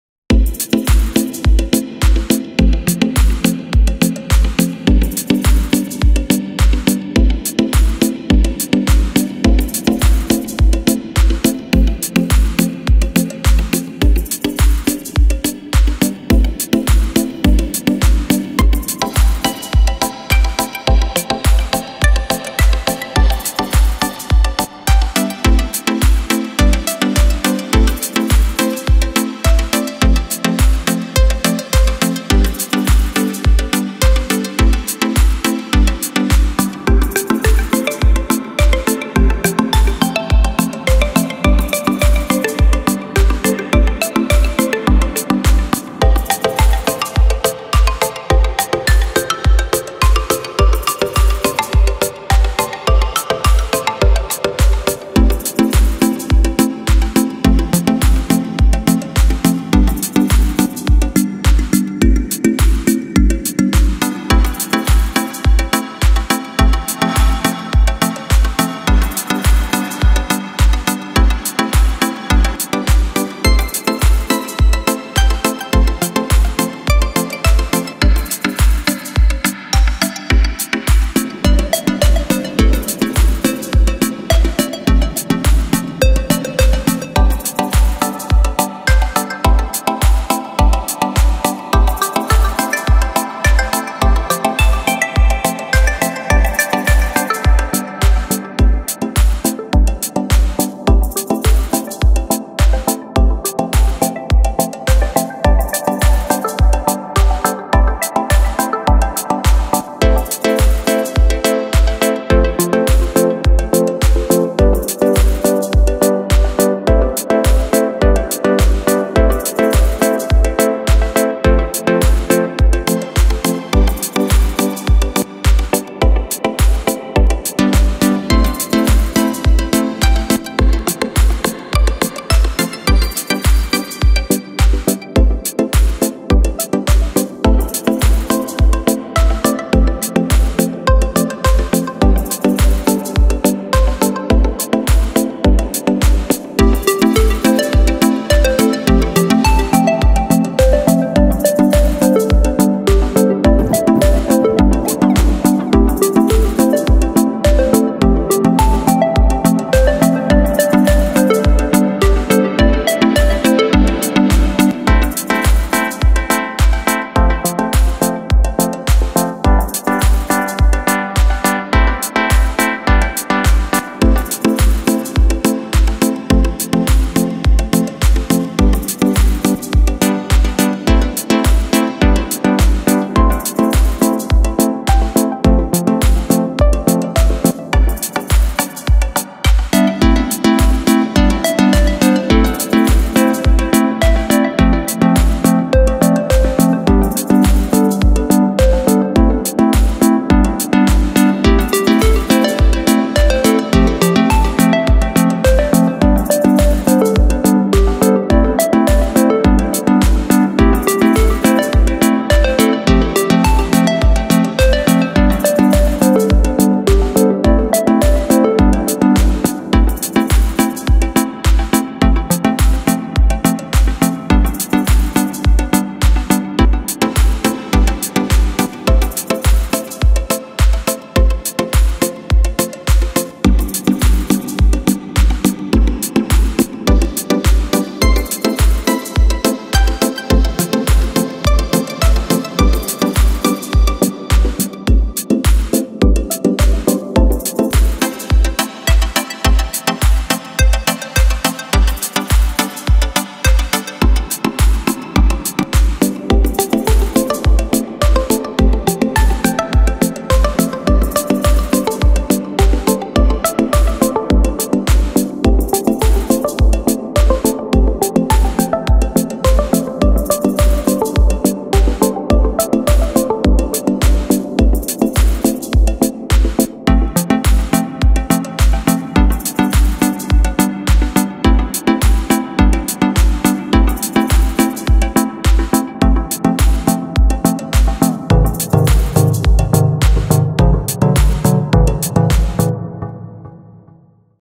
FL Studio 20.5 FLEX Synth - Tropical House song feat synth plucks and presets
In this video I'm showing the pluck sounds available in the new FLEX synth in FL Studio 20.5. This synth sounds awesome and is perfectly suited for EDM and tropical house,